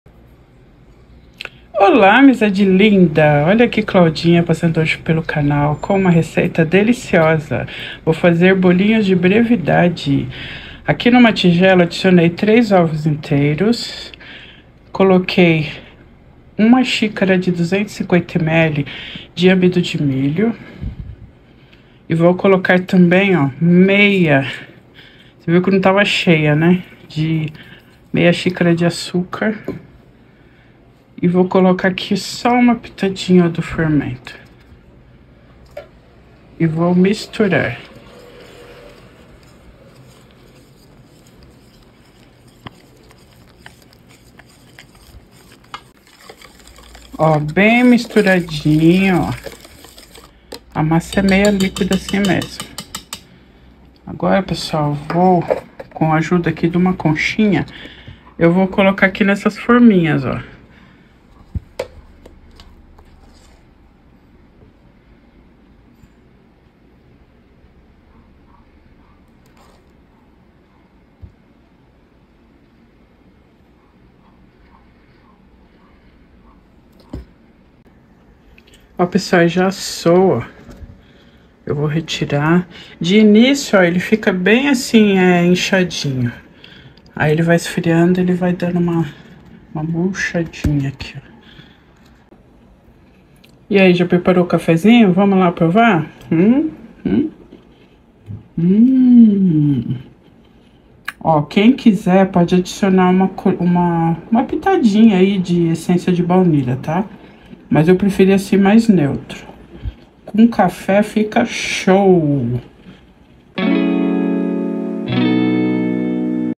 Bolinho De Maisena/Sem Trigo, Sem Glúten e Sem Leite Animal
Publicado em 5 março de 2024

Olá amizade linda 😻

Bolinho sem glúten e sem leite

Ingredientes / medida da xícara 250 ml

3 ovos

1 xícara de amido de milho ( maizena)  = 120 gramas

1/2 xícara de açúcar = 80 gramas ou 20 gramas  adoçante culinário , prove ao seu gosto 😋

1 colher chá fermento químico = 8 gramas

1 colher chá essência de baunilha (opcional)

O modo de preparo está no vídeo…

Avalie o video por gentileza…￼